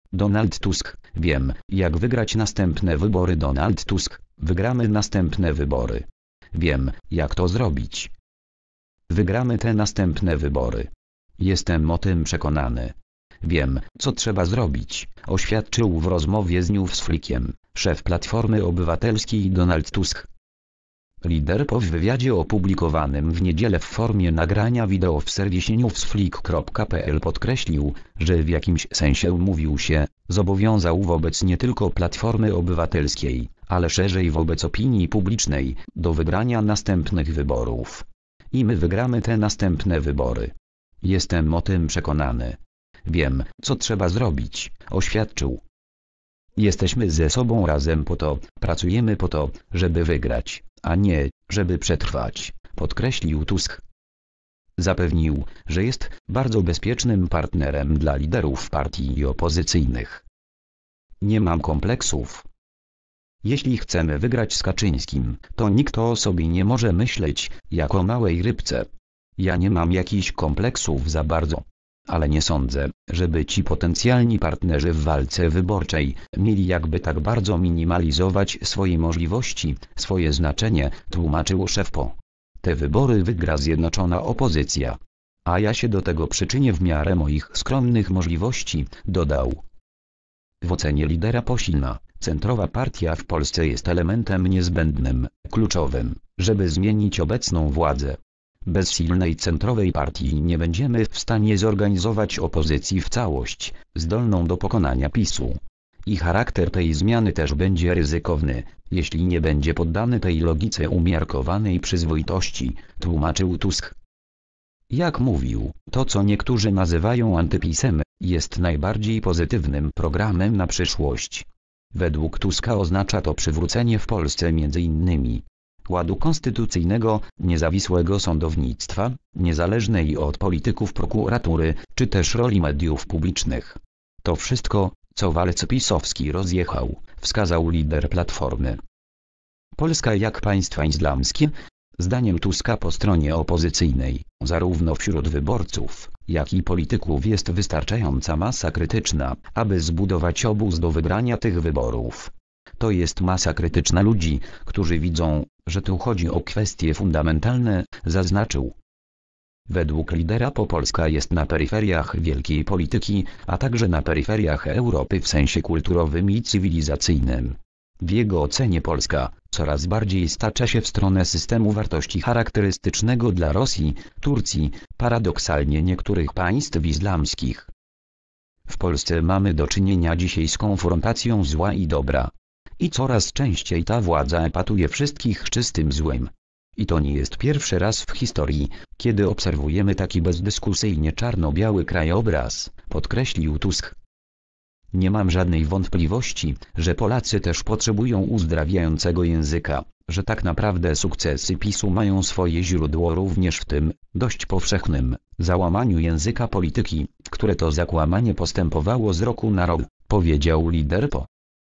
Donald Tusk: Wiem, jak wygrać następne wybory.
Donald Tusk: Wygramy następne wybory. Wiem, jak to zrobić.